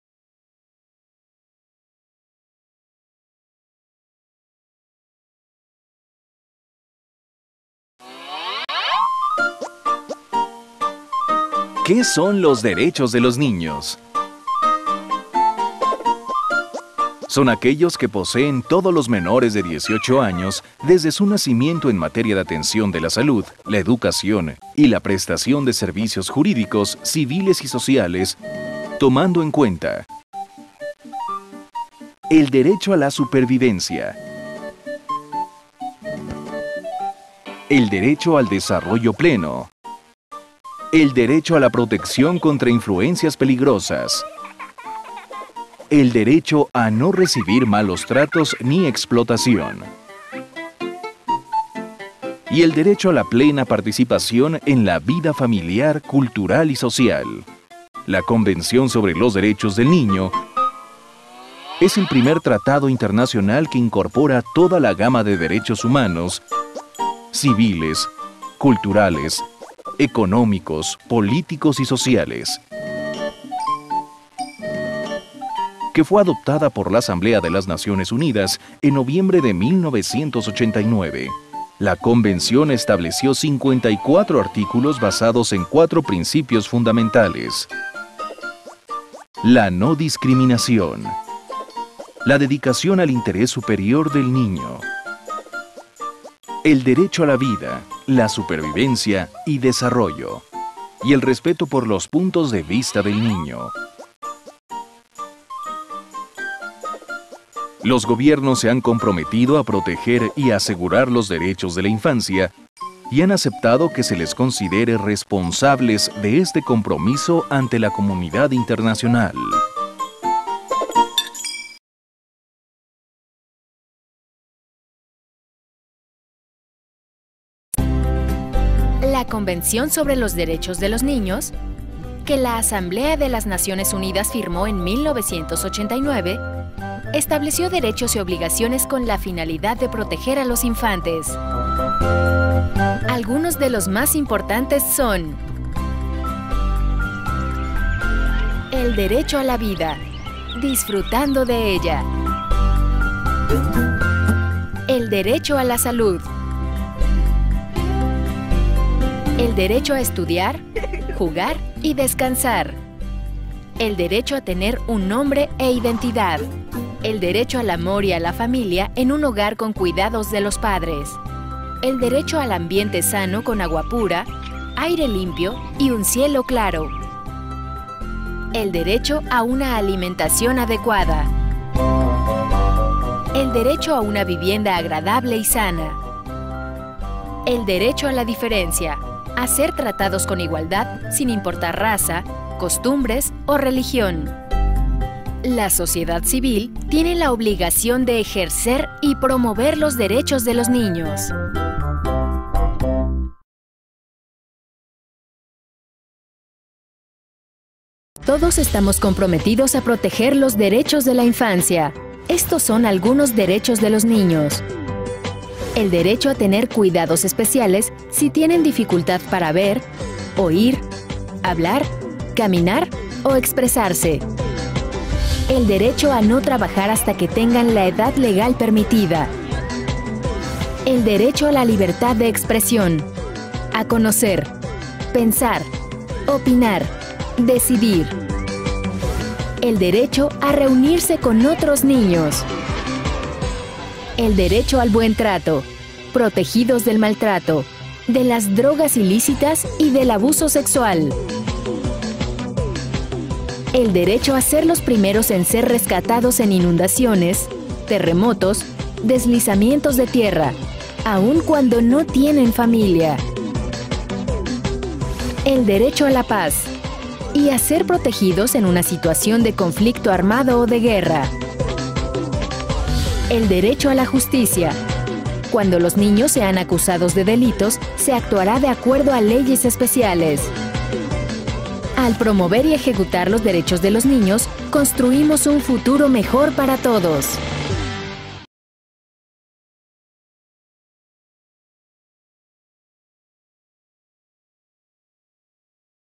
Los derechos de los niños
Los derechos de los niños son todos los que tienen los niños menores de 18 años en materia de salud, educación y prestación de servicios jurídicos, civiles y sociales.

El Gobierno está comprometido para proteger y asegurar los derechos de la infancia.